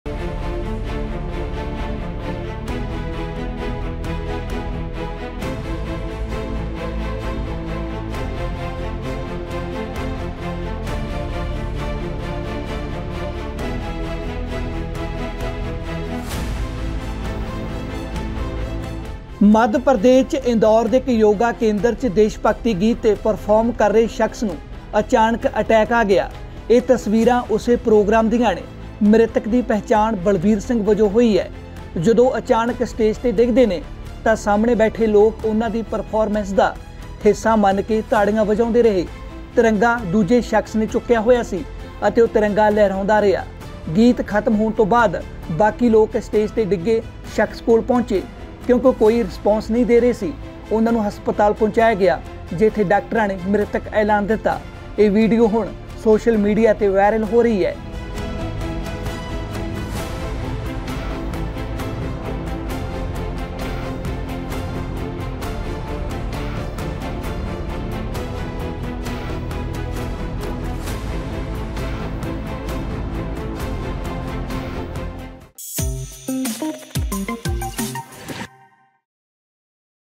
Heart Attack| ਵਿਅਕਤੀ ਨੂੰ ਪਿਆ ਦਿਲ ਦਾ ਦੌਰਾ, ਲੋਕ ਵਜਾਉਂਦੇ ਰਹੇ ਤਾੜੀਆਂ
ਮੱਧ ਪ੍ਰਦੇਸ਼ ਚ ਇੰਦੌਰ ਦੇ ਇੱਕ ਯੋਗਾ ਕੇਂਦਰ ਚ ਦੇਸ਼ਭਗਤੀ ਗੀਤ ਤੇ ਪਰਫੌਮ ਕਰ ਰਹੇ ਸ਼ਖ਼ਸ ਨੂੰ ਅਚਾਨਕ ਅਟੈਕ ਆ ਗਿਆ, ਇਹ ਤਸਵੀਰਾਂ ਉਸੇ ਪ੍ਰੋਗਰਾਮ ਦੀਆਂ ਨੇ, ਮ੍ਰਿਤਕ ਦੀ ਪਹਿਚਾਣ ਬਲਬੀਰ ਸਿੰਘ ਵਜੋਂ ਹੋਈ, ਜਦੋਂ ਉਹ ਅਚਾਨਕ ਸਟੇਜ ਤੇ ਡਿੱਗਦੇ ਨੇ ਤਾਂ ਸਾਹਮਣੇ ਬੈਠੇ ਲੋਕ ਉਨ੍ਹਾਂ ਦੀ ਪਰਫੌਰਮੈਂਸ ਦਾ ਹਿੱਸਾ ਮੰਨ ਕੇ ਤਾੜੀਆਂ ਵਜਾਉਂਦੇ ਰਹੇ, ਤਿਰੰਗਾ ਦੂਜੇ ਸ਼ਖਸ ਨੇ ਚੁੱਕਿਆ ਹੋਇਆ ਅਤੇ ਉਹ ਤਿਰੰਗਾ ਲਹਿਰਾਉਂਦਾ ਰਿਹਾ, ਗੀਤ ਖ਼ਤਮ ਹੋਣ ਤੋਂ ਬਾਅਦ ਬਾਕੀ ਲੋਕ ਸਟੇਜ ਤੇ ਡਿੱਗੇ ਸ਼ਖਸ ਕੋਲ ਪਹੁੰਚੇ, ਕਿਉਂਕਿ ਉਹ ਕੋਈ ਰਿਸਪੌਂਸ ਨਹੀਂ ਦੇ ਰਹੇ ਸੀ, ਉਨ੍ਹਾਂ ਨੂੰ ਹਸਪਤਾਲ ਪਹੁੰਚਾਇਆ ਗਿਆ ਜਿਥੇ ਡਾਕਟਰਾਂ ਨੇ ਉਨ੍ਹਾਂ ਨੂੰ ਮ੍ਰਿਤਕ ਐਲਾਨ ਦਿੱਤਾ, ਇਹ ਵੀਡੀਓ ਹੁਣ ਸੋਸ਼ਲ ਮੀਡੀਆ ਤੇ ਵਾਇਰਲ ਹੋ ਰਹੀ ਐ,

ABP ਸਾਂਝਾ ਸਾਰੇ ਸੋਸ਼ਲ ਮੀਡੀਆ ਪਲੇਟਫਾਰਮਾਂ ਤੇ ਉਪਲੱਬਧ ਹੈ। ਤੁਸੀਂ ਸਾਨੂੰ ਫੇਸਬੁੱਕ, ਟਵਿੱਟਰ, ਕੂ, ਸ਼ੇਅਰਚੈੱਟ ਅਤੇ ਡੇਲੀਹੰਟ 'ਤੇ ਵੀ ਫੋਲੋ ਕਰ ਸਕਦੇ ਹੋ।